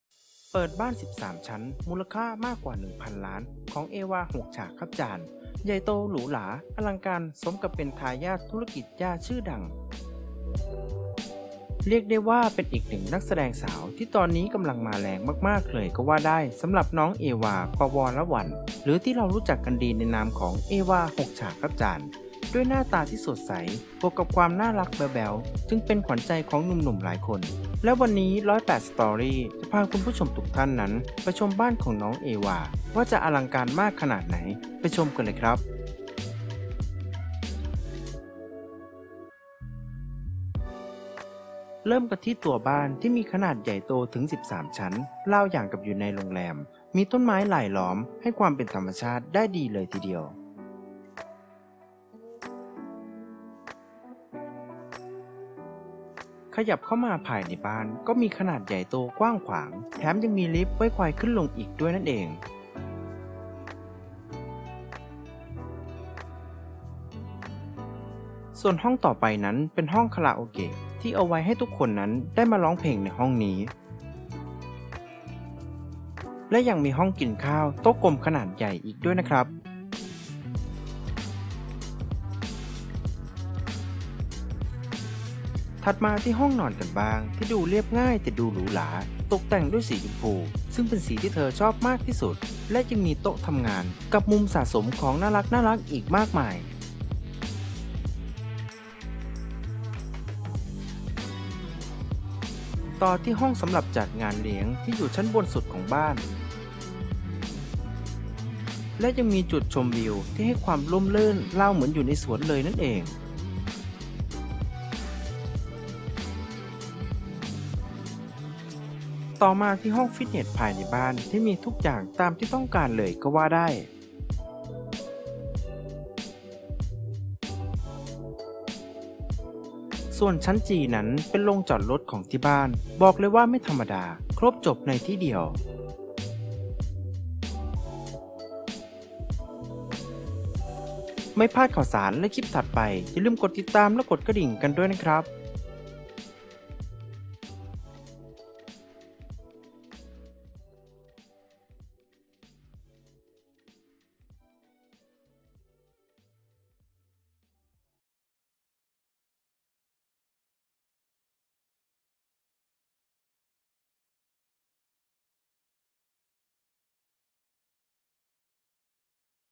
เปิดบ้าน13 ชั้น ของ"เอวา หกฉากครับจารย์"มูลค่ามากกว่า 1000 ล้าน แบบทุกซอกทุกมุม
เปิดบ้าน13 ชั้น ของ"เอวา หกฉากครับจารย์"มูลค่ามากกว่า 1000 ล้าน แบบทุกซอกทุกมุมใหญ่โตหรูหรา อลังการ สมกับเป็นทายาทธุรกิจยาชื่อดัง